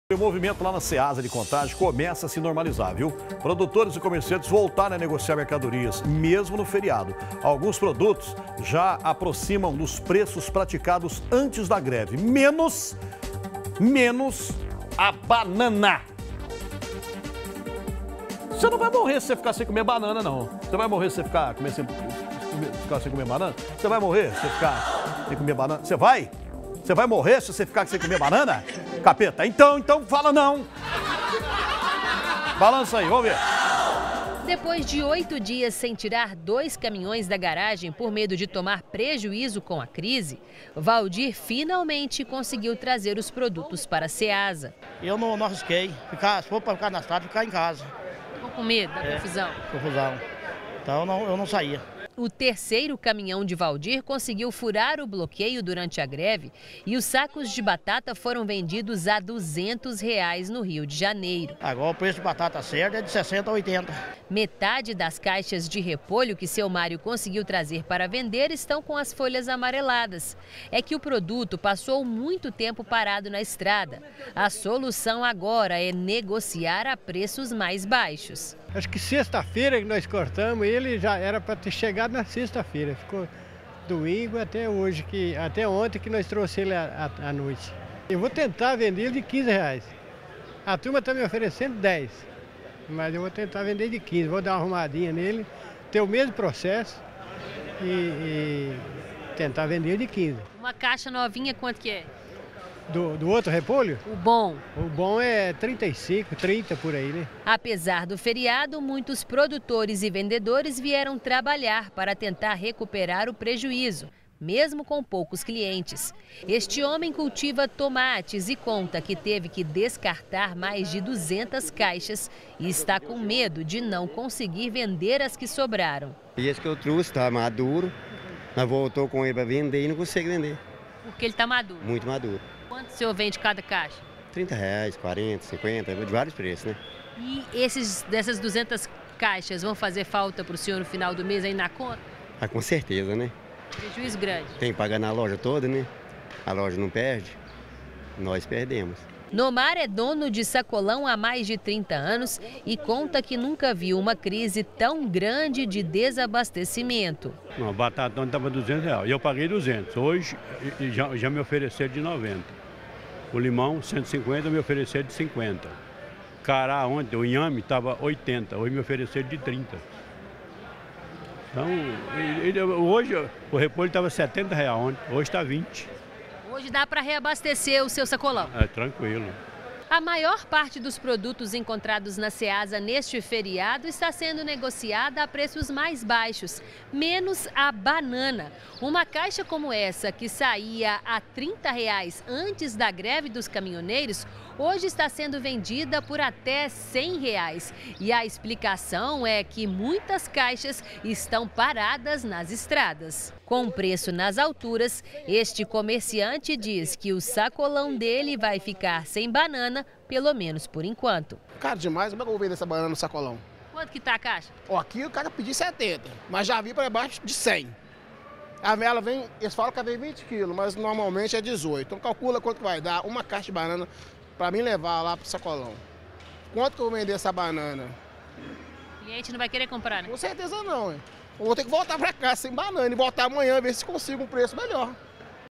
Preço médio da banana sobe na Ceasa de Contagem (MG)
O movimento na Ceasa de Contagem começa a se normalizar. Produtores e comerciantes voltaram a negociar mercadorias, nesta quinta-feira (31), mesmo sendo feriado. O preço de muitos produtos já está próximo ao que era praticado antes da greve dos caminhoneiros, menos o valor da banana.